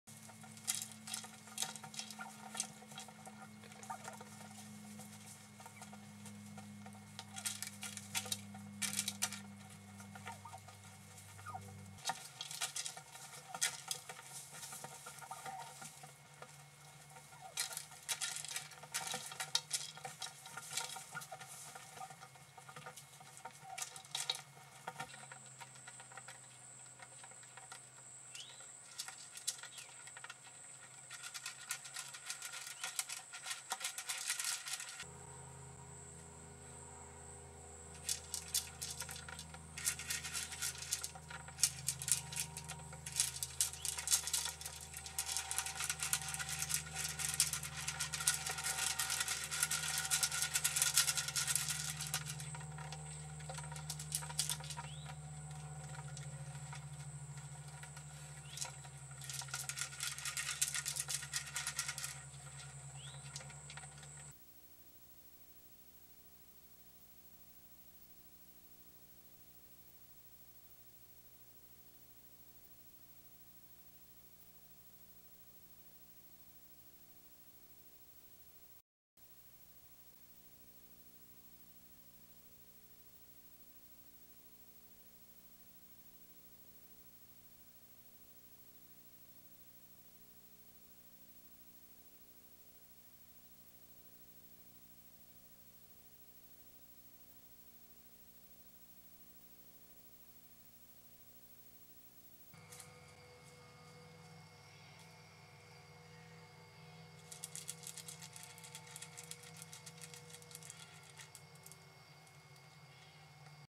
Turkeys, Deer, and a Gator.avi
Some various clips of Turkeys several Deer and a Gator.